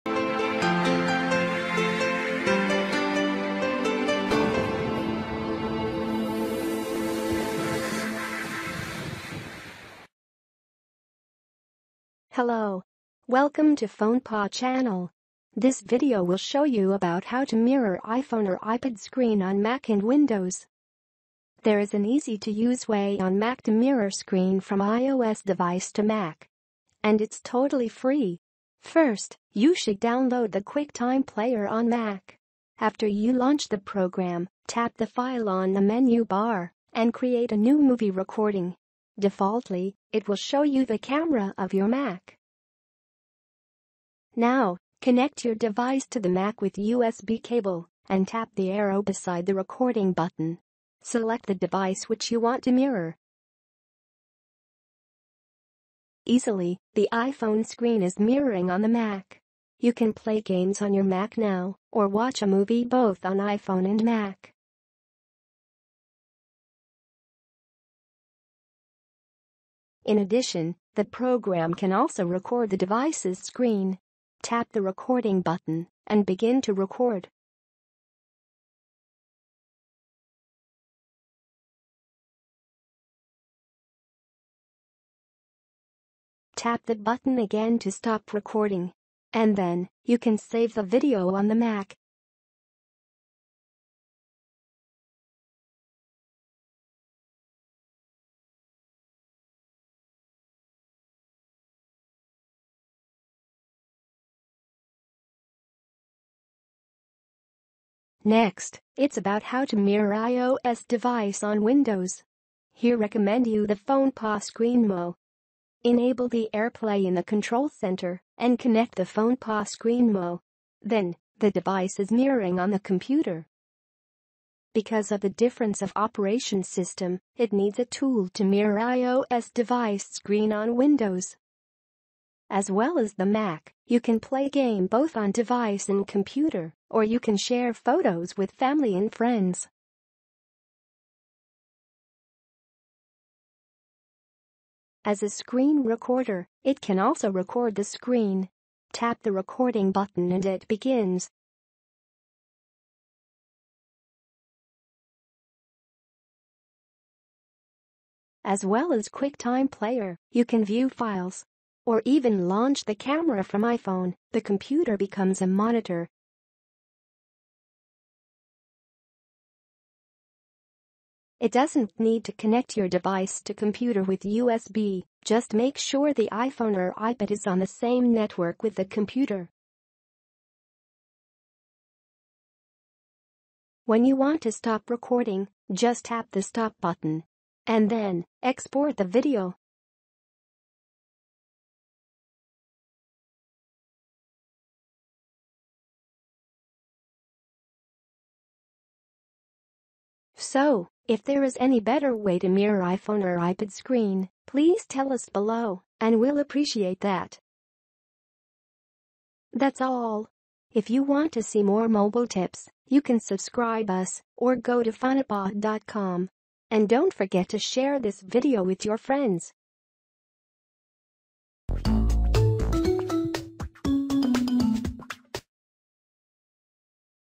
How to Mirror iPhone on Mac and PC (QuickTime Player and ScreenMo)
How to Mirror iPhone Screen/Display to Mac or Windows: Want to have a display on your Mac or PC that looks just like your iPhone? You will be able to do so without any problems in a matter of minutes with no jailbreak need. It is useful to play a movie you have on your iPhone on your computer screen for example or to want photos on a larger screen.